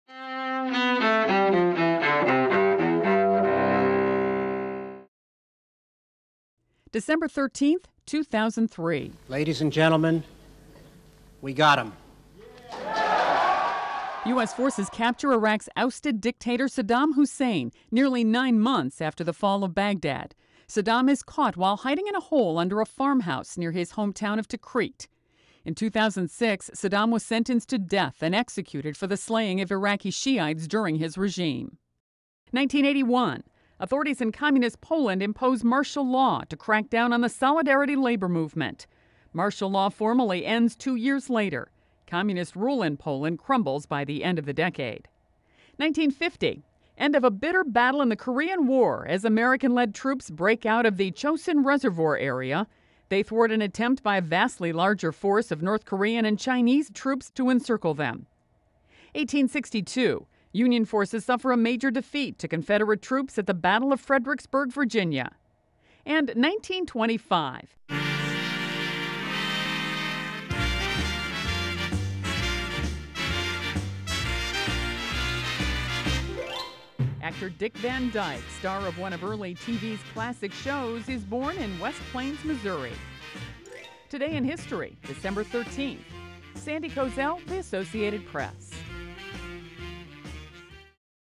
Today in History for December 13th
U.S. forces capture Iraq's ousted dictator Saddam Hussein;  Authorities in communist Poland impose martial law;  Union forces suffer defeat at the Battle of Fredericksburg, Virginia; Actor Dick Van Dyke is born. (Dec. 13)